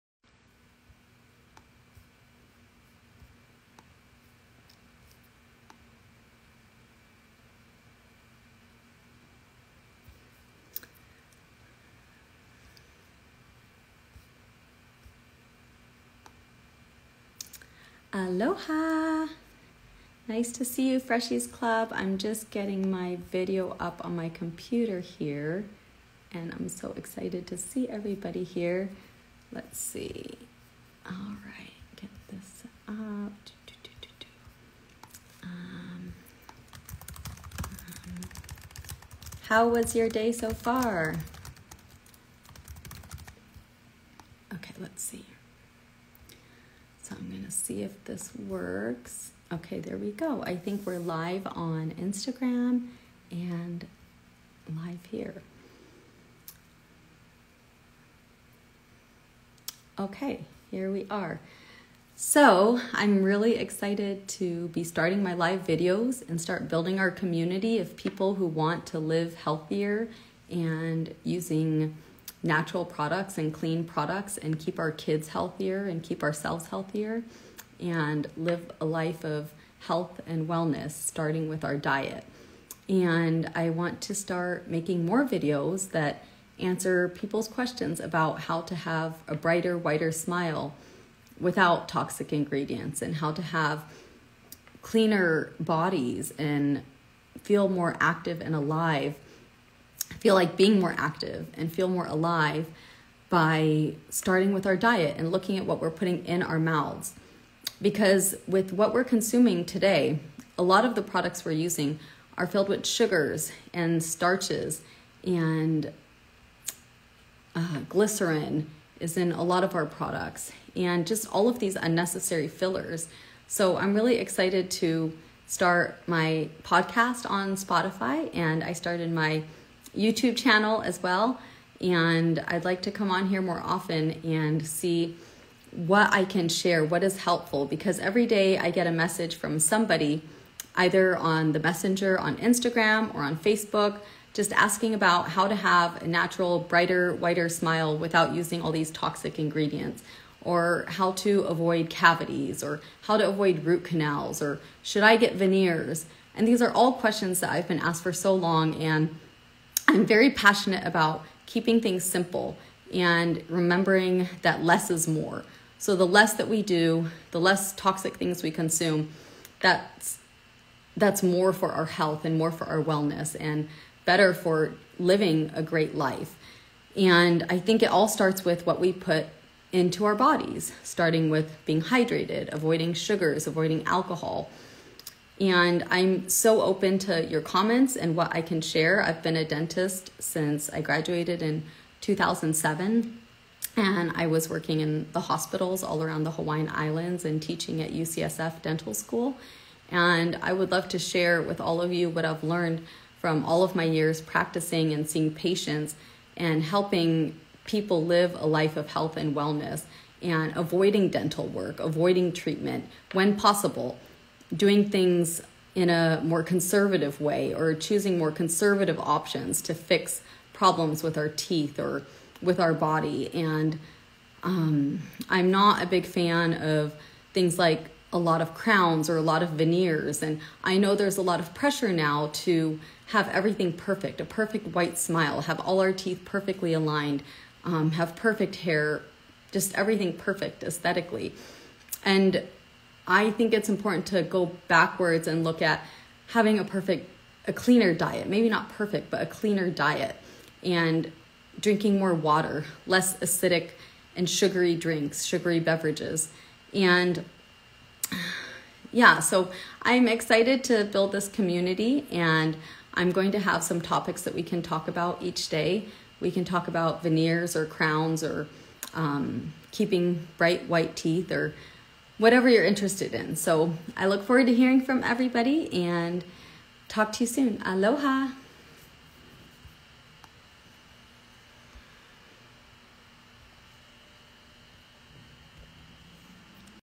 Starting live videos + building a community 🫶🏽✨🙌🏽 〰️ LET ME KNOW 〰️ if there are any topics you guy
Starting live videos + building a community 🫶🏽✨🙌🏽

〰️ LET ME KNOW 〰️ if there are any topics you guys would like for me to chat on! Leave a comment or send a DM!